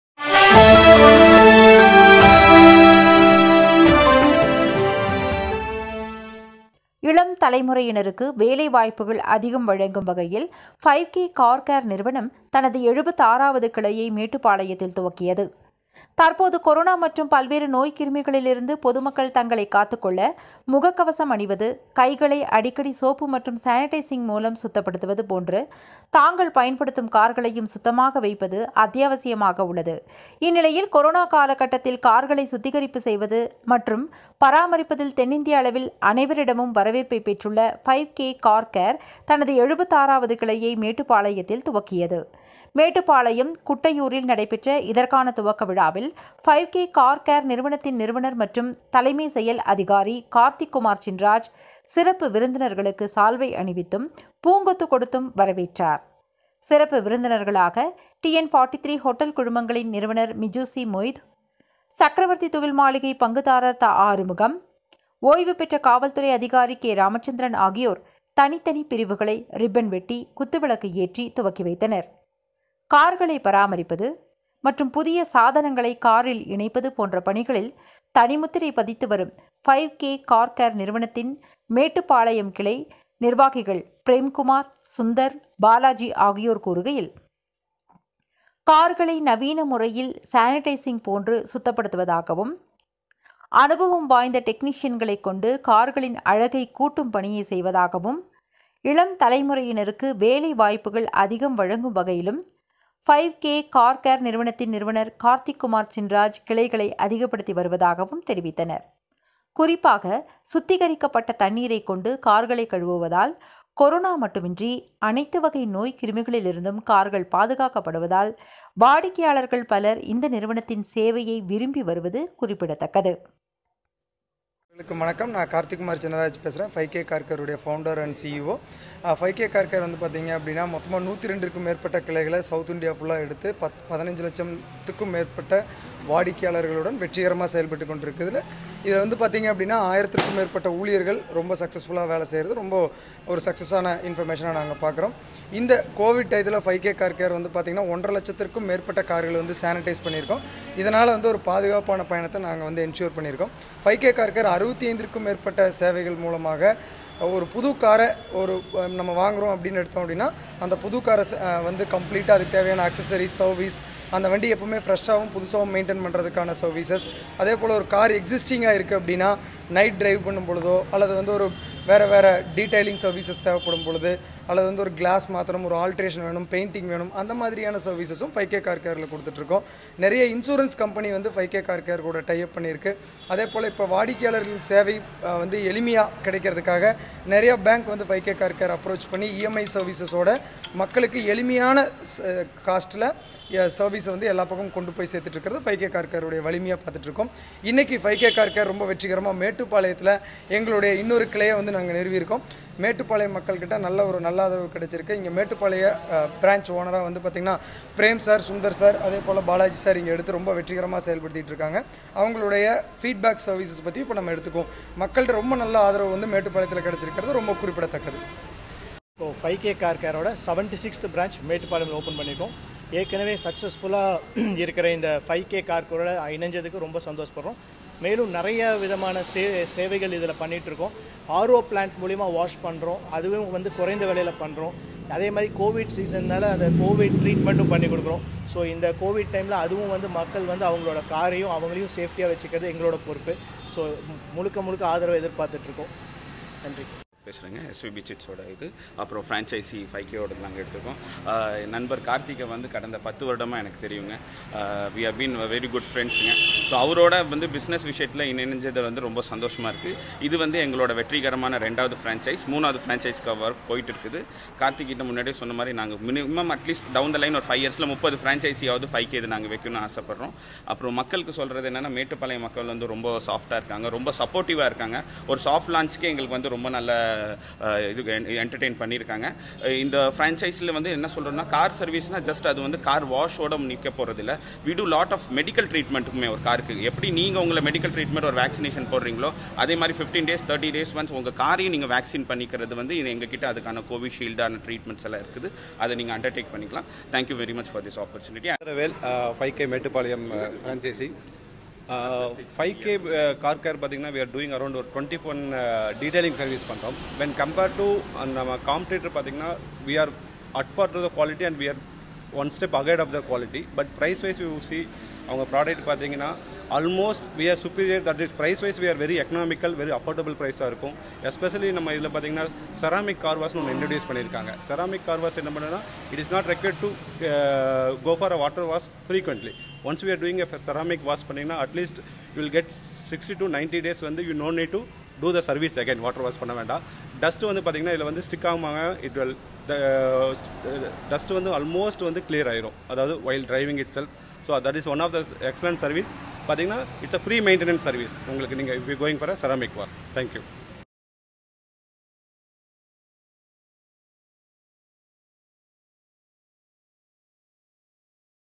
5கே கார் கேர் நிறுவனம் 76 வது கிளை மேட்டுப்பாளையத்தில் துவக்கம்
இளம் தலைமுறையினருக்கு வேலை வாய்ப்புகள் அதிகம் வழங்கும் வகையில், 5 கே கார் கேர் நிறுவனம் தனது 76 வது கிளையை மேட்டுப்பாளையத்தில் துவக்கியது

தற்போது கொரோனா மற்றும் பல்வேறு நோய் கிருமிகளில் இருந்து  பொதுமக்கள் தங்களை காத்து கொள்ள முக கவசம் அணிவது கைகளை அடிக்கடி சோப்பு மற்றும் சானிட்டைசிங் மூலம் சுத்தப்படுத்துவது போன்று தங்கள் பயன்படுத்தும் கார்களையும் சுத்தமாக வைப்பது அத்தியாவசியமாக உள்ளது...இந்நிலையில் கொரோனா கால கட்டத்தில் கார்களை சுத்திகரிப்பு செய்வது மற்றும் பராமரிப்பதில் தென்னிந்திய அளவில் அனைவரிடம் வரவேற்பை பெற்றுள்ள 5 கே கார் கேர் தனது 76 வது கிளையை மேட்டுப்பாளையத்தில் துவக்கியது.மேட்டுப்பாளையம், குட்டையூரில்,நடைபெற்ற இதற்கான துவக்க விழாவில்,5 கே கார் கேர் நிறுவனத்தின் நிறுவனர் மற்றும் தலைமை செயல் அதிகாரி, கார்த்திக்குமார் சின்ராஜ் சிறப்பு விருந்தினர்களுக்கு சால்வை அணிவித்தும் பூங்கொத்து கொடுத்தும் வரவேற்றார்.சிறப்பு விருந்தினர்களாக டி.என் 43 ஹோட்டல் குழுமங்களின் நிறுவனர் மிஜு.சி. மொய்து, சக்கரவர்த்தி துகில் மாளிகை பங்குதாரர் தஆறுமுகம்,ஓய்வு பெற்ற காவல்துறை அதிகாரி  K.இராமச்ந்திரன் ஆகியோர் தனித்தனி பிரிவுகளை ரிப்பன் வெட்டி, குத்துவிளக்கு ஏற்றி துவக்கி வைத்தனர். கார்களை பராமரிப்பது,மற்றும் புதிய சாதனங்களை காரில் இணைப்பது போன்ற பணிகளில் தனி முத்திரை பதித்து வரும்,5 கே கார் கேர் நிறுவனத்தின் மேட்டுப்பாளையம் கிளை நிர்வாகிகள் பிரேம்குமார், சுந்தர், பாலாஜி ஆகியோர் கூறுகையில், கார்களை நவீன முறையில் சானிட்டைசிங் போன்று சுத்தபடுத்துவதாகவும், அனுபவம் வாய்ந்த டெக்னீசியன்களை கொண்டு கார்களின் அழகை கூட்டும் பணியை செய்வதாகவும்,  இளம் தலைமுறையினருக்கு வேலை வாய்ப்புகள் அதிகம் வழங்கும் வகையில், 5 கே கார் நிறுவனத்தின் நிறுவனர் கார்த்திக்குமார் சின்ராஜ் கிளைகளை அதிகபடுத்தி வருவதாக தெரிவித்தனர். குறிப்பாக சுத்திகரிக்கப்பட்ட தண்ணீரை கொண்டு கார்களை கழுவுவதால்,கொரோனா மட்டுமின்றி அனைத்து வகை நோய் கிருமிகளில் இருந்தும் கார்கள் பாதுகாக்கபடுவதால்,வாடிக்கையாளர்கள் பலர் இந்த நிறுவனத்தின் சேவையை விரும்பி வருவது குறிப்பிடதக்கது…